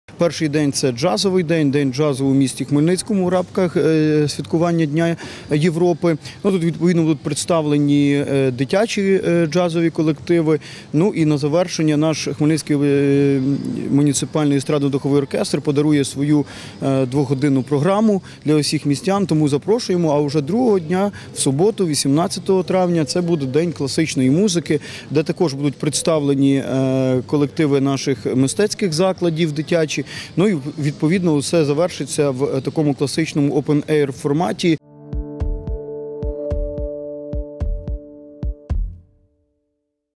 Дні Європи відзначать у Хмельницькому
Дні Європи відзначать у Хмельницькому. Триватиме захід два дні – 17 та 18 травня. Про це розповів начальник управління культури та туризму Хмельницької міської ради Артем Ромасюков. За його словами, у перший день заходу лунатимуть джазові композиції у виконані колективів міста. Наступного дня – гратимуть класичну музику.